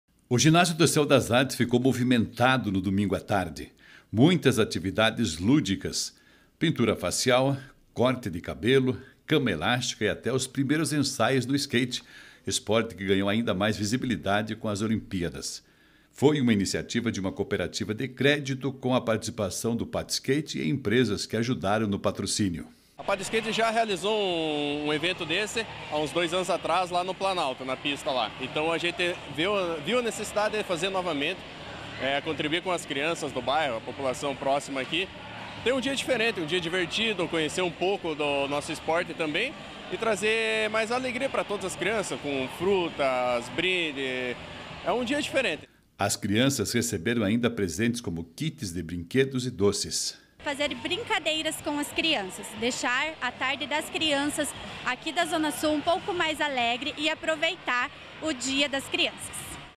Ginásio do CEU das Artes fica movimentado com as atividades alusivas ao Dia das Crianças
Eventos: Na tarde de domingo (13), o Ginásio do CEU das Artes ficou movimentado com as atividades voltadas ao público infantil.